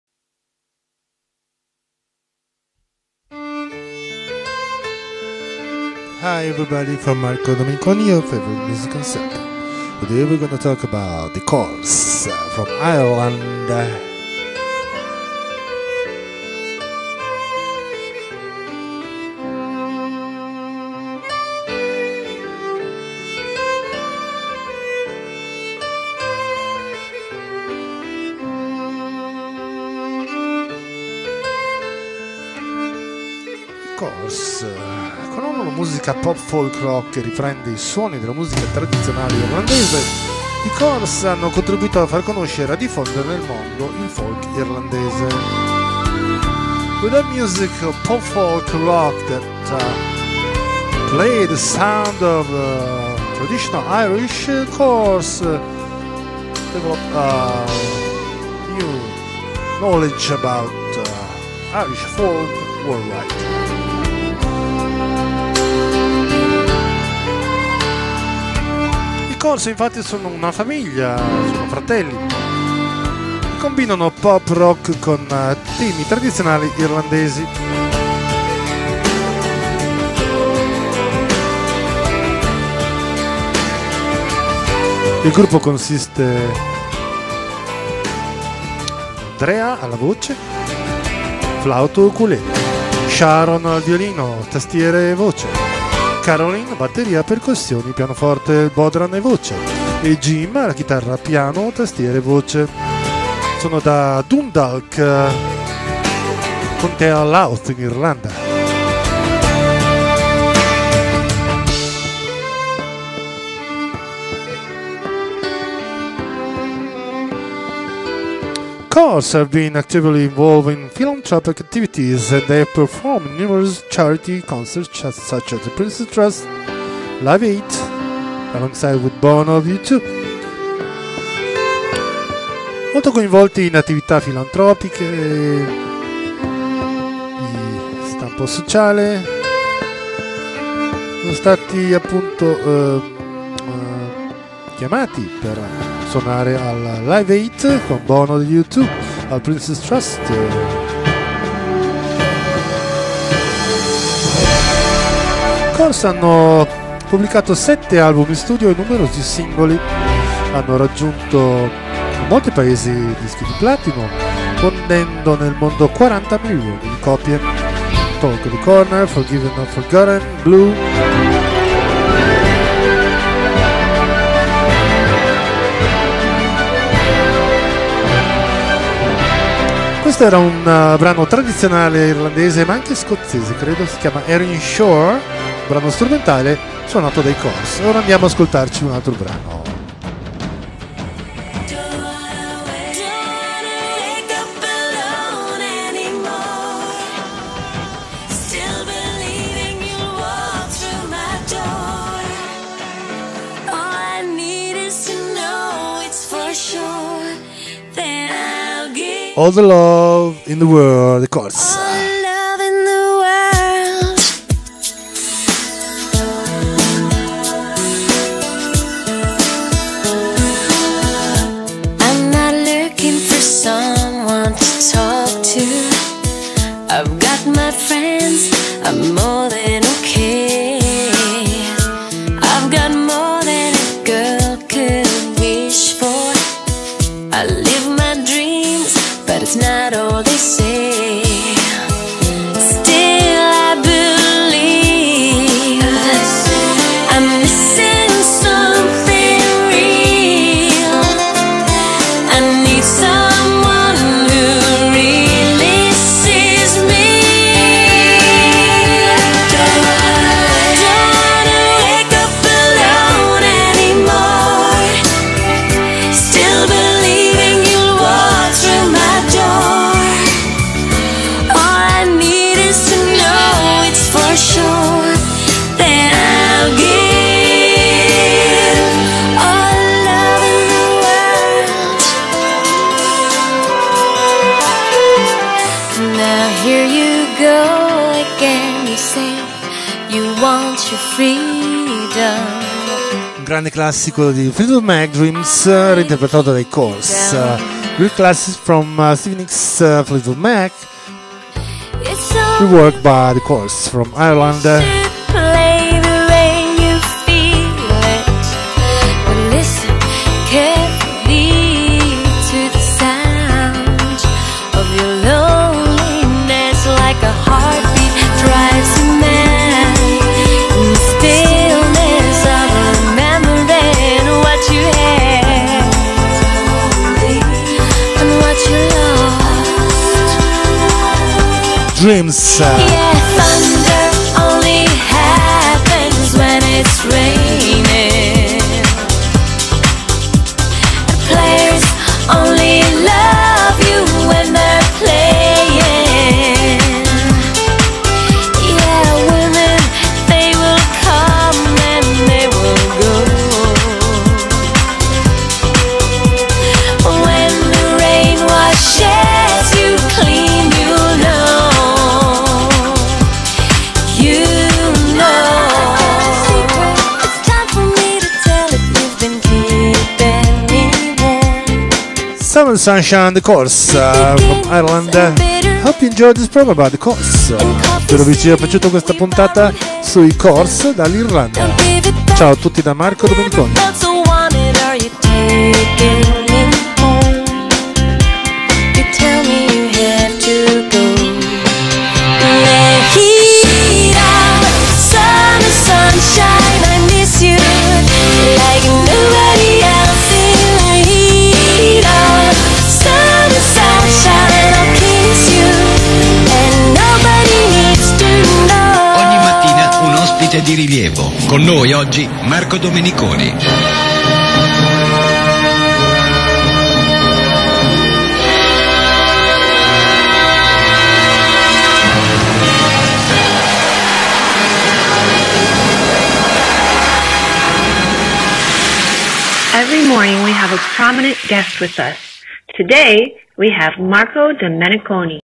NON SOLO VINILE | The Corrs
Dalla Casa della Musica, con Marco Domeniconi, una guida attraverso l'universo musicale.

Il vostro consulente musicale vi augura buona giornata con The Corrs.